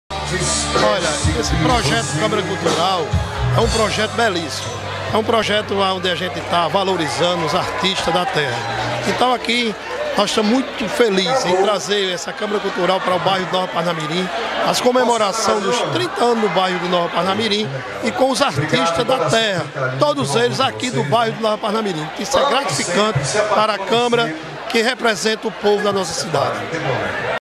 Câmara Cultural 2019: Vereador Irani Guedes
Em comemoração aos 30 anos de Nova Parnamirim, a Câmara de Municipal realizou nesta sexta-feira (24), pela primeira vez na localidade o projeto ‘Câmara Cultural’ – que tem o objetivo de valorizar a cultura local e sua diversidade de ritmos e estilos.